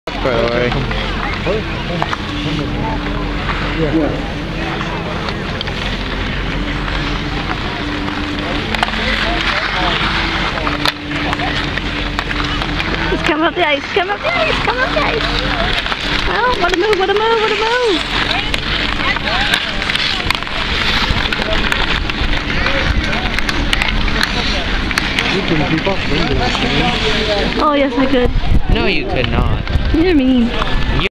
womens teams in the world pond hockey championschips in plaster rock ,new brunswick
women had 18 teams in the world pond hockey championships in plaster rock new Brunswick.